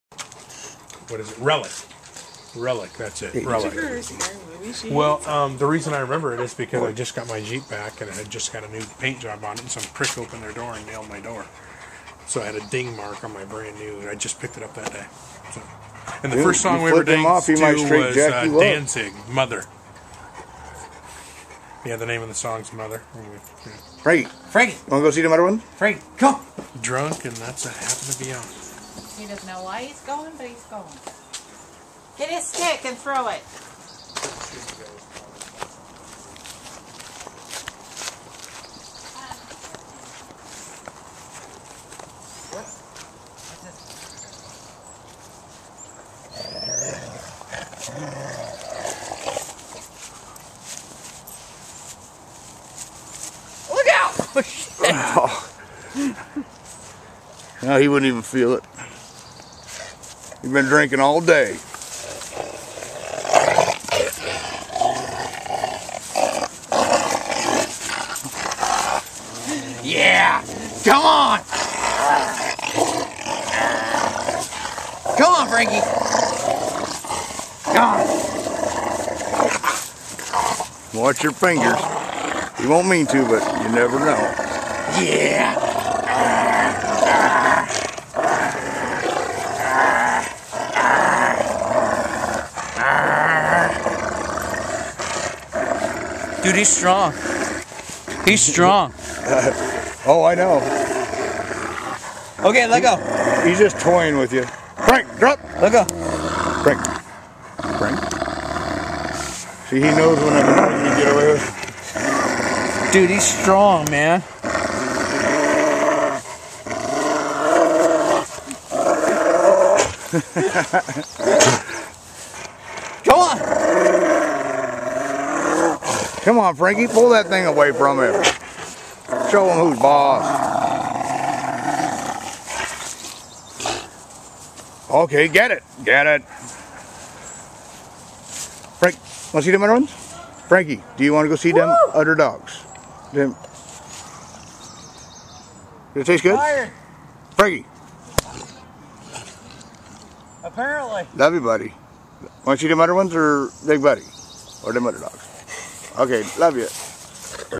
Rottweiler tug o war. Master of Fire... Wood...
Having a few cocktails on a Sunday afternoon with my best friend Frank... Frank, kickin Larry"s ass, and you don't have to tell me my back yard looks like white trash (at the moment) I know. I'm working on it! So to the inevitable negative commenter I just want to throw out a pre-emptive ; Blow Me :)